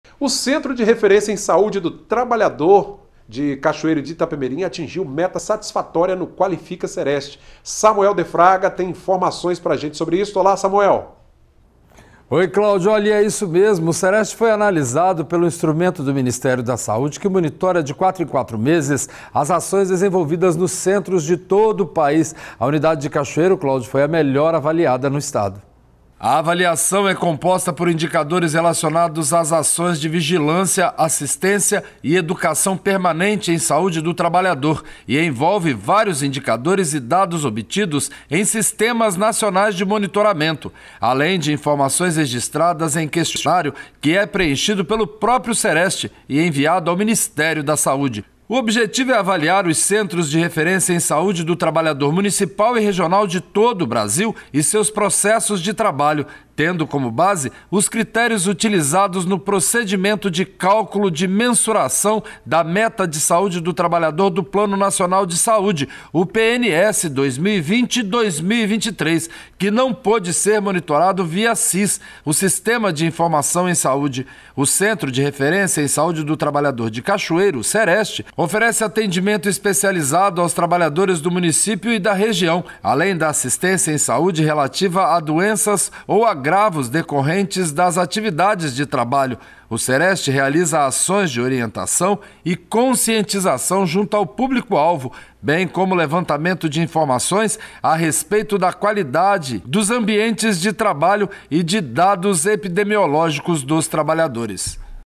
META SATISFATÓRIA: Cerest atinge marca no Qualifica CEREST, instrumento monitorado pelo Min.da Saúde
Apresentador: Cláudio Alves / Samuel de Fraga
Repórter: Samuel de Fraga